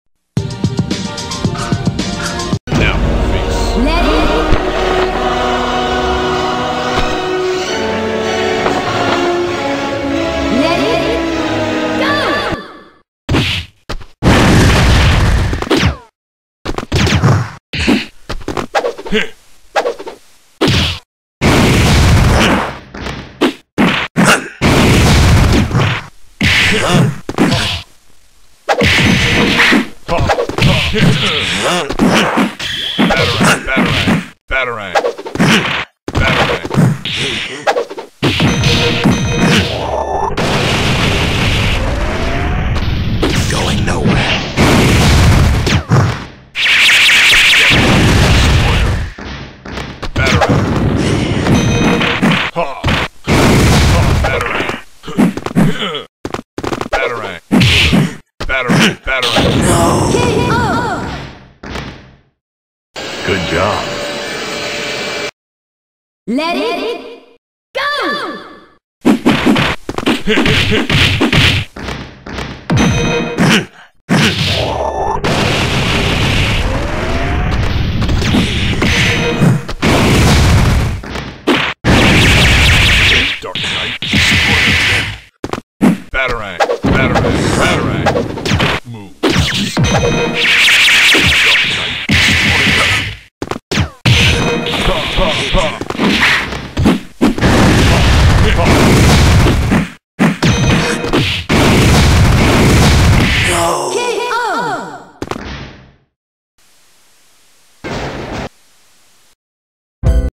Batman vs Rorschach
Who will win this requested matchup between The Dark Knight and Rorschach.  Any matchup ideas leave in the comments.